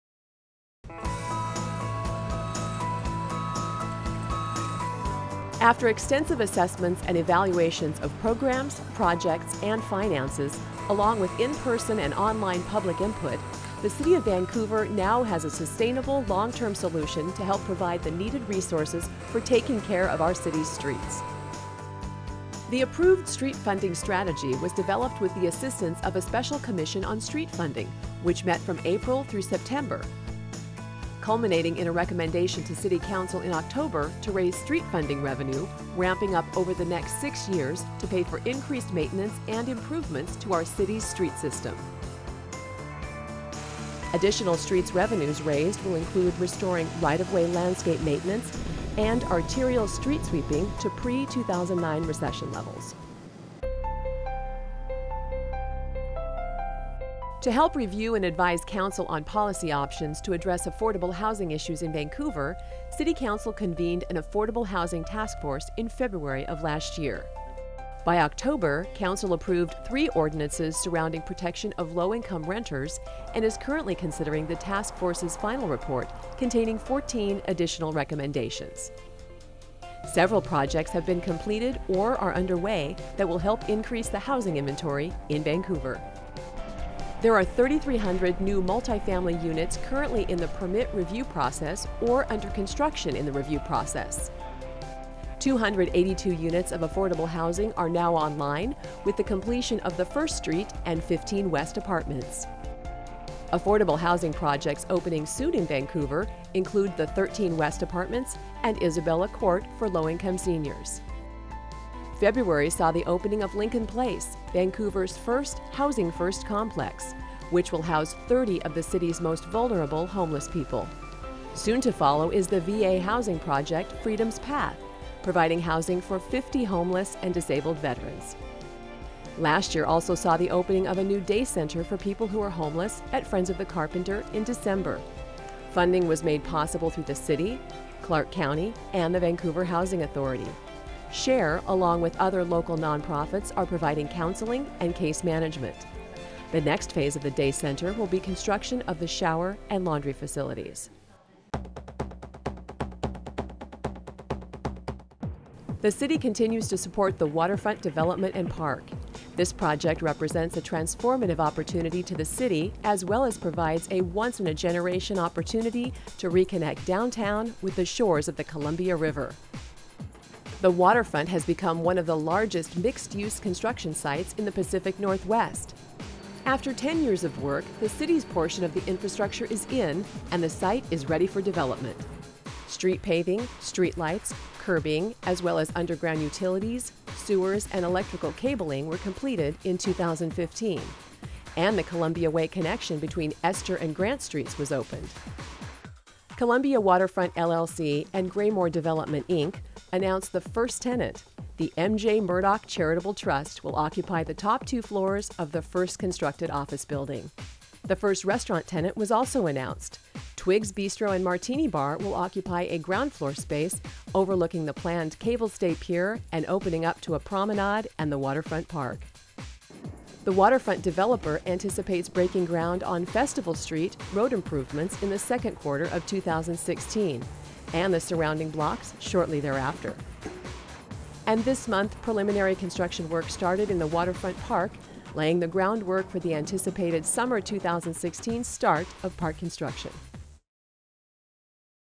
2016 State of the City Update
A 2016 update on the City street funding strategy, affordable housing initiatives and the development of the downtown Vancouver waterfront.